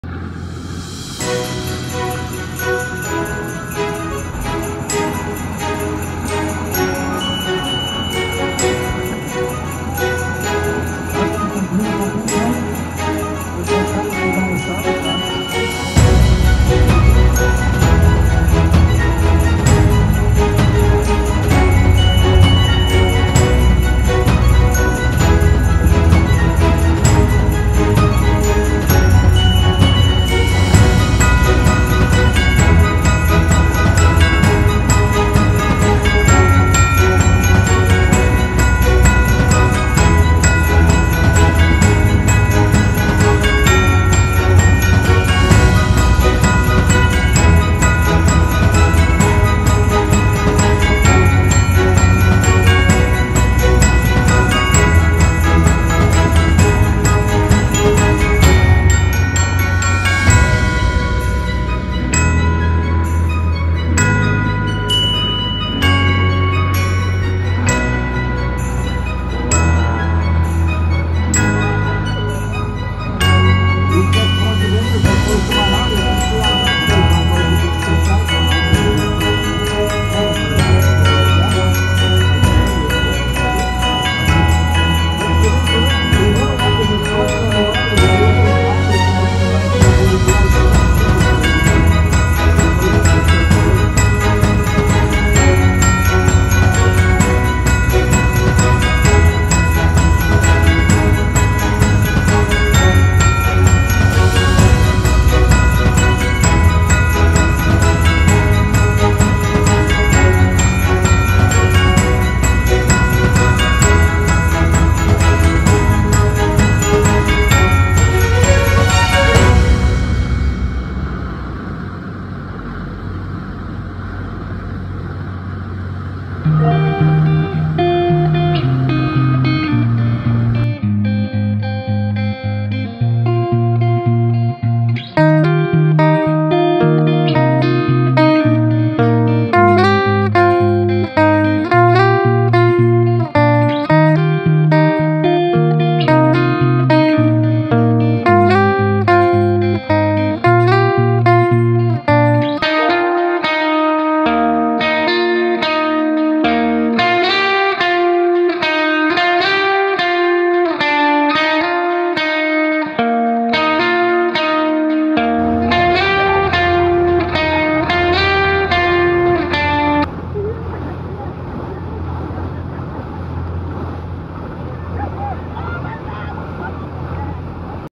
Dolphins in the Charleston Harbor
Enjoy the tranquility of dolphins swimming in the bow wave of the Carolina Belle as we sail around the Charleston Harbor.